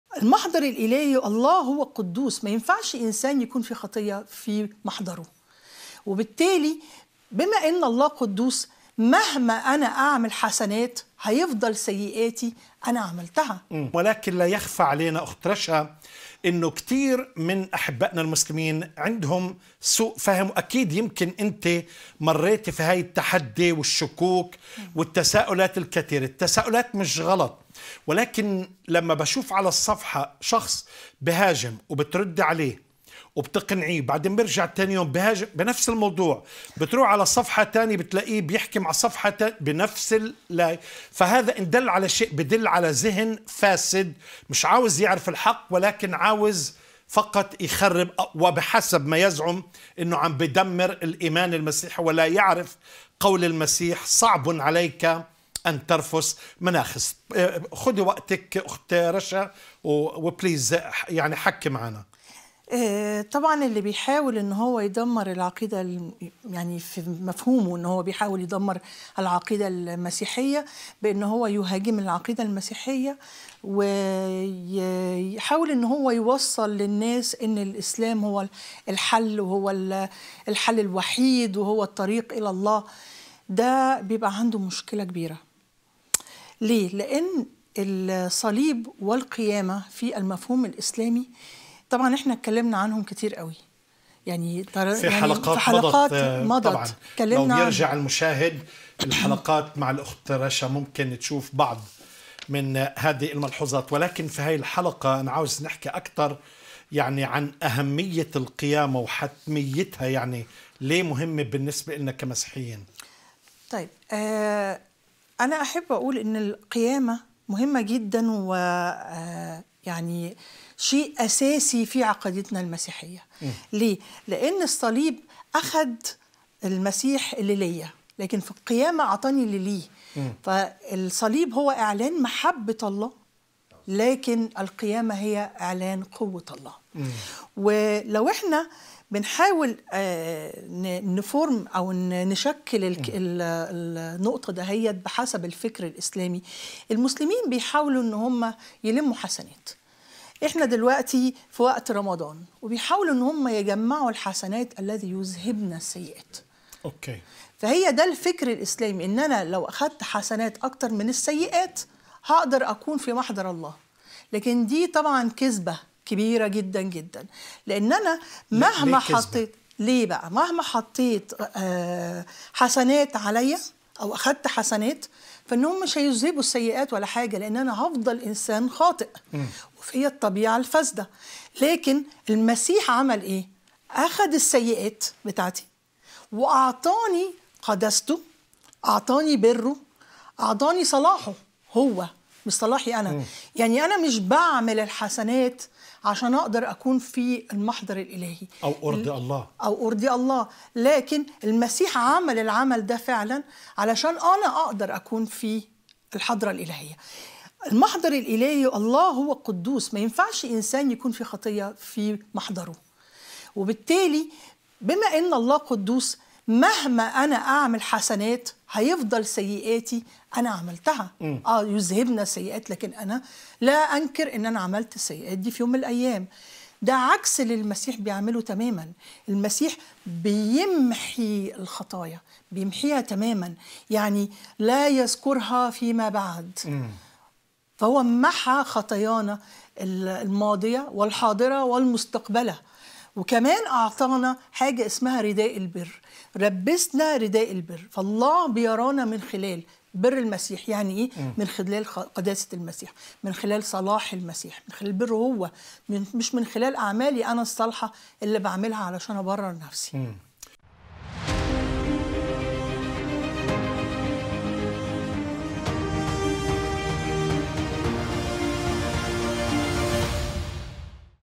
02- هل حقاً أن الحسنات يذهبن السيئات ؟
👈هل حقاً أن الحسنات يذهبن السيئات؟
👈كيف يمكن للإنسان أن يتبرر أمام الله؟

📌يمكنك التواصل معنا بصورة شخصية عن طريق رسائل الواتساب👇
WhatsApp الواتساب:    🆆🅷🅰🆃🆂🅰🅿🅿